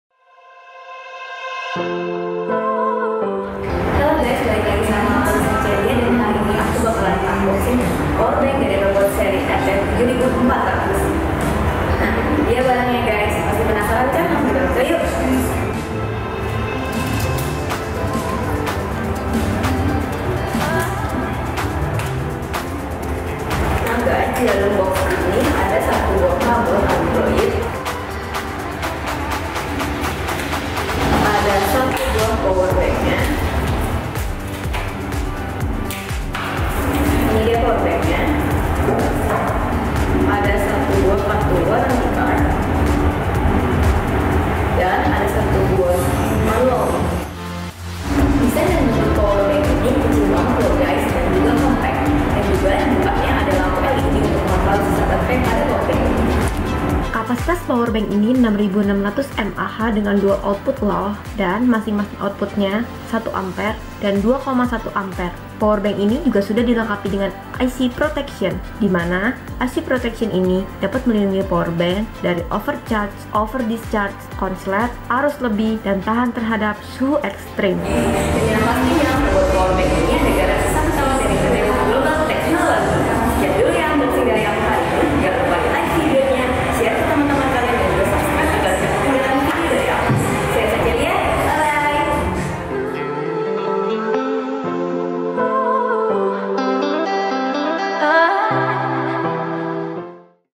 UNBOXING - ROBOT Powerbank RT7400
Holiday is coming guys! Kalian mau jalan-jalan tapi takut smartphone lowbat? ROBOT powerbank RT7400 ini solusinya.

Powerbank RT7400 ini memiliki Body Arc Design yang memberikan kenyamanan ketika digenggam dengan satu tangan. Powerbank ROBOT ini berkapasitas 6600mAh dan memiliki 2 USB Port dengan output pertama sebesar 1A dan output kedua sebesar 2,1A. Selain sudah support fast charging, terdapat tampilan digital juga loh untuk memudahkan kamu mengetahui sisa daya powerbank-nya.

Sama seperti powerbank ROBOT lainnya, Powerbank RT7400 ini sudah dilengkapi dengan Intelligent Protection yang mampu mencegah over-charge, over-discharge, over-voltage, over-current, over-temperature, short-circuit dan recovery protection sehingga aman saat digunakan untuk peralatan elektronik kesayangan kamu.

Kamu bisa mendapatkan powerbank ROBOT RT7400 ini di booth VIVAN PRJ JiExpo Kemayoran Hall C2 dan D2. Ada banyak penawaran menarik loh!

Pastikan kalian membeli produk ROBOT dengan garansi resmi PT WOOK GLOBAL TECHNOLOGY.

ROBOT